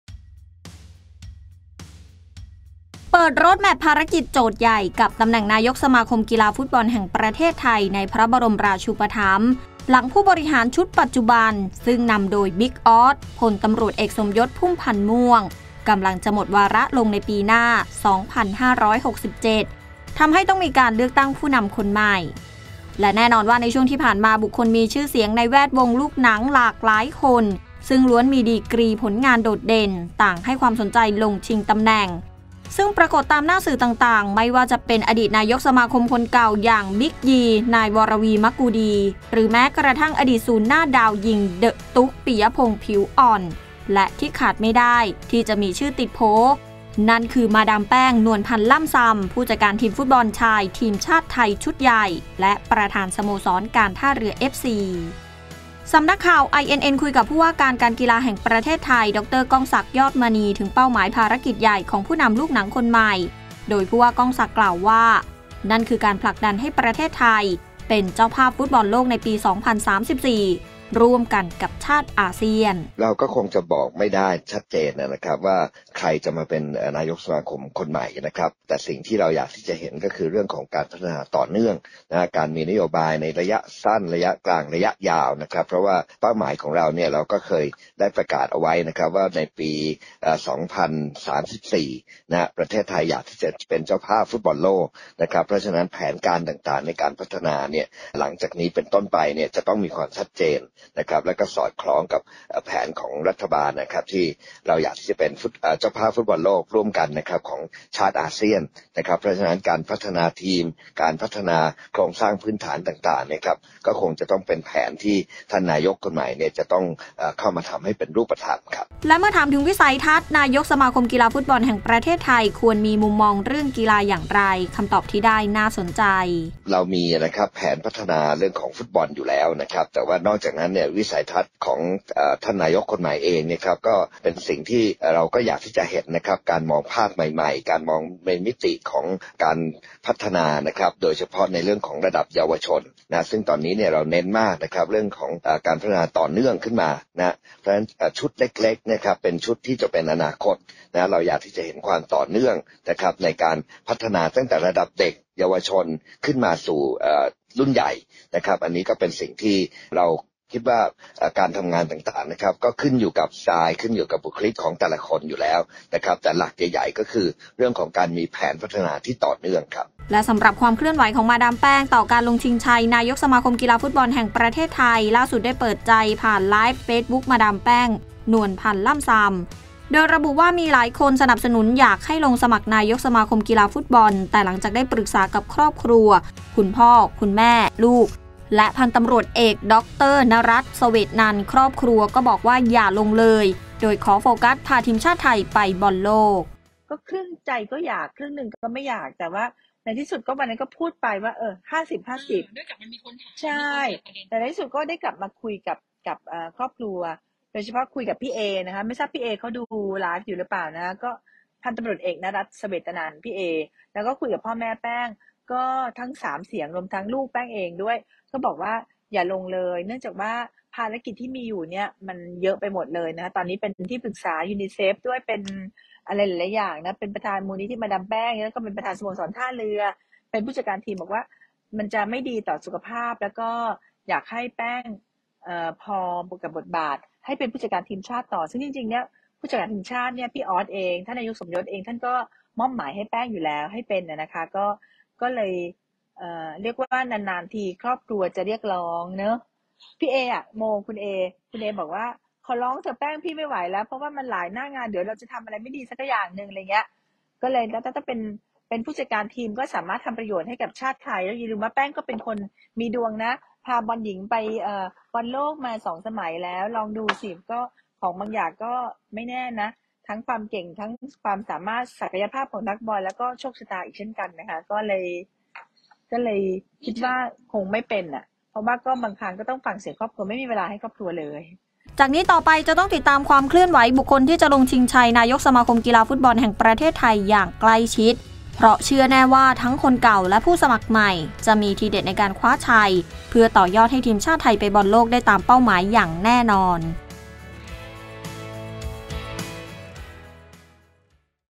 นายกฯคนไหน จะพาไทยไปบอลโลก? : 04-06-66 | iNN Top Story
เปิดโรดแมปภาระกิจโจทย์ใหญ่ กับตำแหน่ง นายกสมาคมกีฬาฟุตบอลแห่งประเทศไทย ในพระบรมราชูปถัมภ์ หลังผู้บริหารชุดปัจจุบันซึ่งนำโดย "บิ๊กอ๊อด-พล.ต.อ.สมยศ พุ่มพันธุ์ม่วง" กำลังจะหมดวาระลงในปีหน้า 2567 ทำให้ต้องมีการเลือกตั้งผู้นำคนใหม่